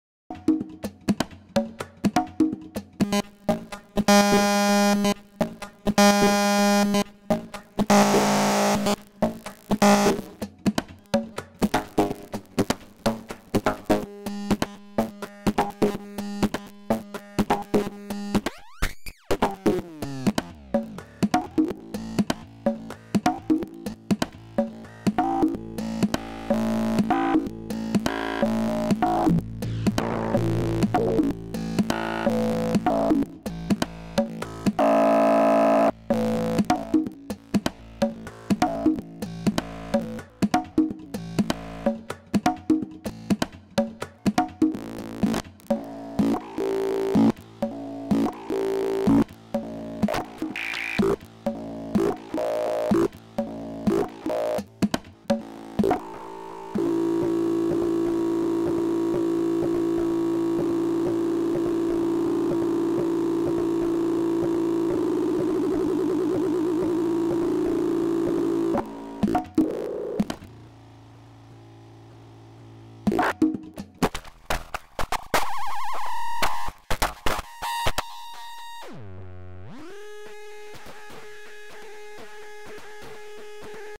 Glitching with buffer 2
Generator/Devices/Buffer module - samples a buffer on each trigger signal coming to its gate input and plays it in a loop. It may be used to create some cool glitches.

Connections:
Sampler output -- Buffer audio input
Sampler output - Filter - Buffer gate input  (sample itself is used to retrigger buffer, on zero crossing, a filter is added in between for some warbles)